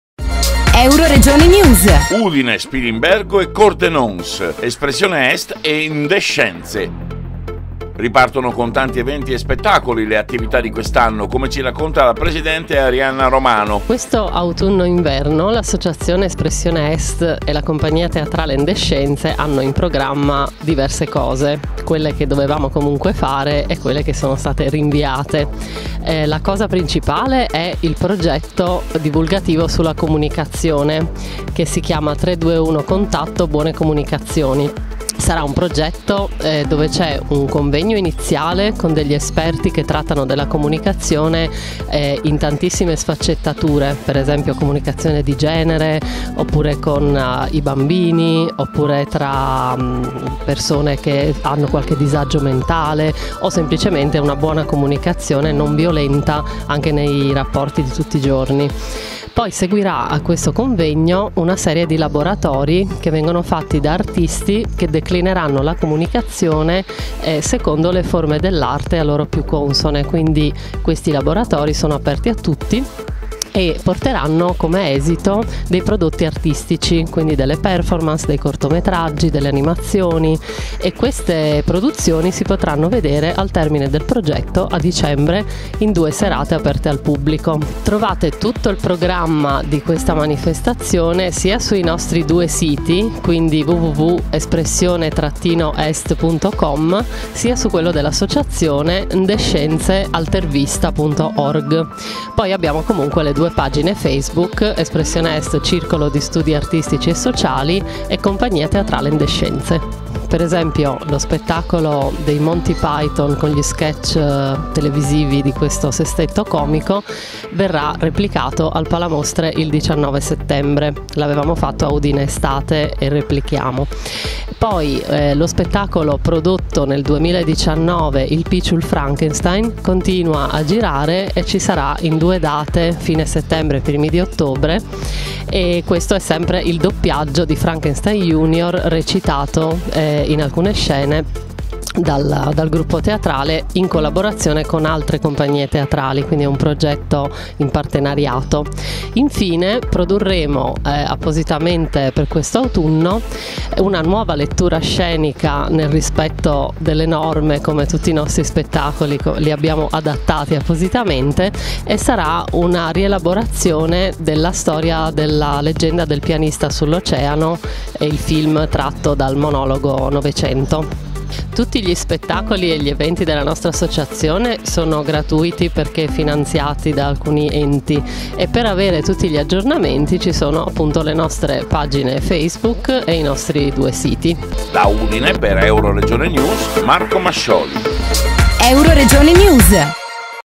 UDINE, SPILIMBERGO e CORDENONS - ESPRESSIONE EST E NDESCENZE
Ripartiranno alla grande con tanti eventi e spettacoli tenuti chiusi nei cassetti a causa del Lockdown. Già dal 19 settembre 2020 le attività culturali di Espressione Est con dieci nuove iniziative tra rappresentazioni teatrali, incontri culturali e laboratori aperti al pubblico, tutte gratuite. Giunta al suo ventiseiesimo anno di vita, con programmi e realizzazioni improntate nell’ottica di un apertura al contatto con il prossimo, l’associazione continua a portare avanti l’obiettivo di porre l’accento sulle diverse modalità espressive di tutte le forme creative coinvolgendo e facendo formazione a sempre più fruitori.
Un centinaio sinora le attività intraprese in ambiti che spaziano dalla scrittura alle arti visive con mostre, iniziative multimediali, teatro, musica dal vivo, poesia, pubblicazioni periodiche, convegni, laboratori.
Le attività di quest’anno, con tutte iniziative a ingresso libero, ma su prenotazione (numero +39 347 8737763, e-mail o ) partiranno sabato 19 settembre con il primo degli spettacoli della Compagnia Teatrale Ndescenze. Al Teatro Palamostre di Udine sarà messa in scena alle 21 la commedia brillante in atto unico “Deja Vu - mi ricorda qualcosa…” spettacolo con copione realizzato ex novo, tradotto e adattato dai filmati originali del programma televisivo sui Monthy Python.
Altra commedia brillante sarà rappresentata domenica 27 settembre alle 17 presso la sede del Circolo Nuovi Orizzonti: “Il Piçul Frankenstein” è una trasposizione della nota pellicola di Mel Brooks, nella realtà friulana. Terza iniziativa della compagnia teatrale Ndescenze avrà luogo domenica 25 ottobre alle 18 sempre al .